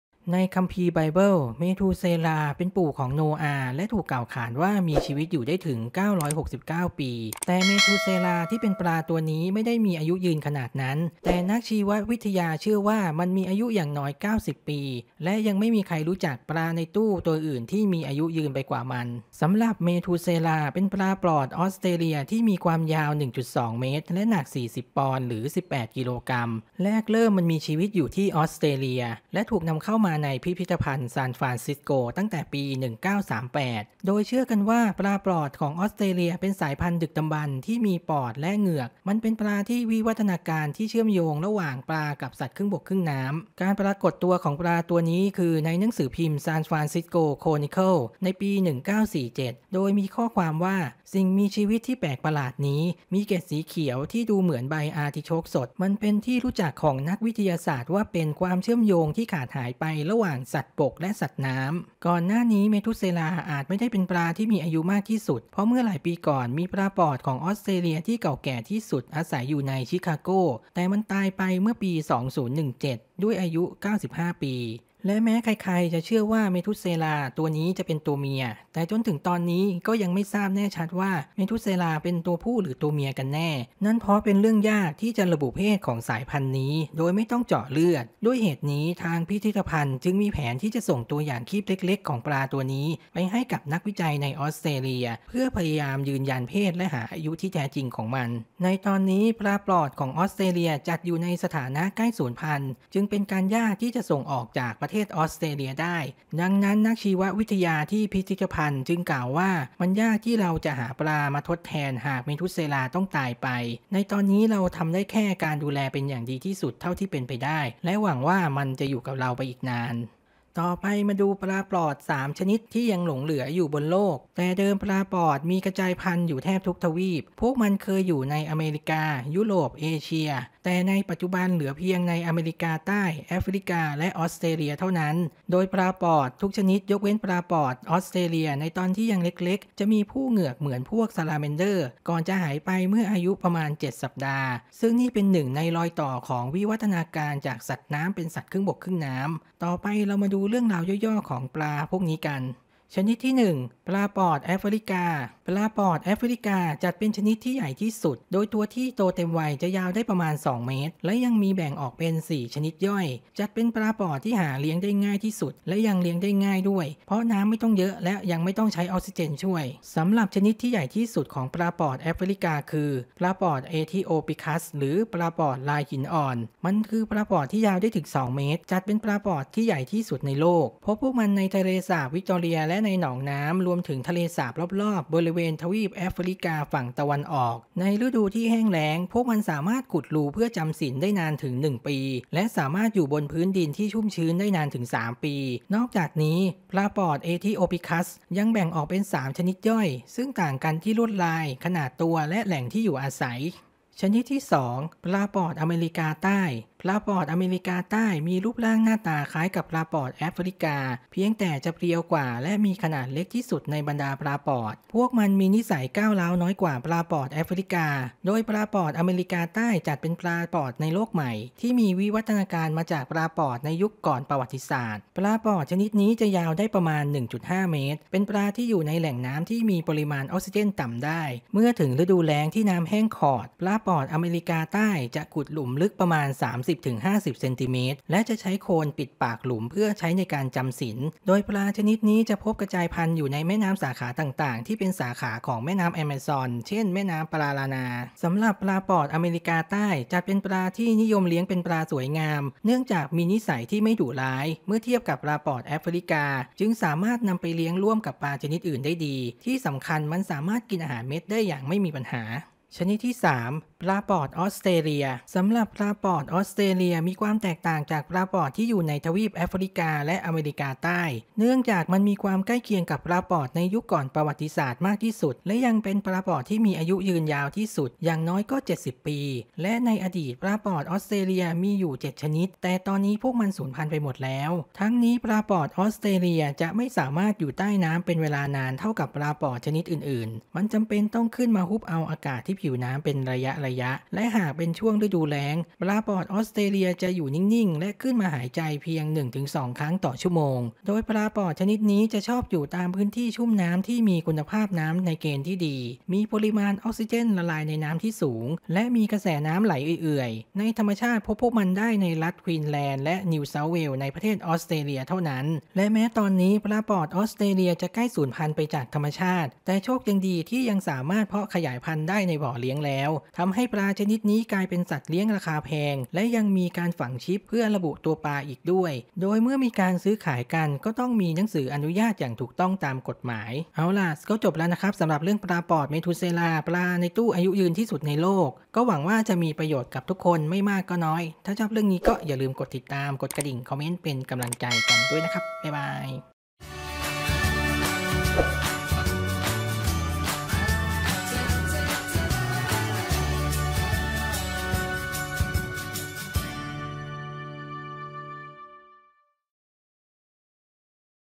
ปลาปอด 'เมธูเสลาห์' ปลาในตู้อายุยืนที่สุดในโลก กับปลาปอด 3 ชนิดสุดท้าย
มาดูเรื่องราวน่ารู้ของปลาปอดกัน ...ไม่อยากพลาดเรื่องดีๆ กดติดตามกดกระดิ่งกันนะ